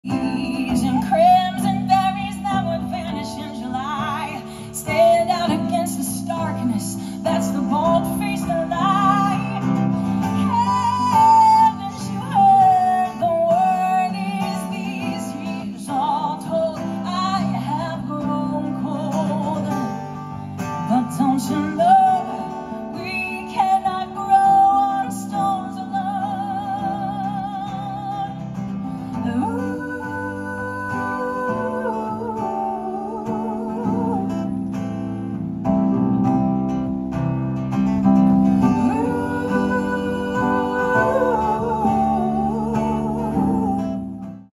Jess Merritt and the Understorey live at Sonic Lunch 2023
Live at Liberty Plaza in downtown Ann Arbor on July 27, 2023.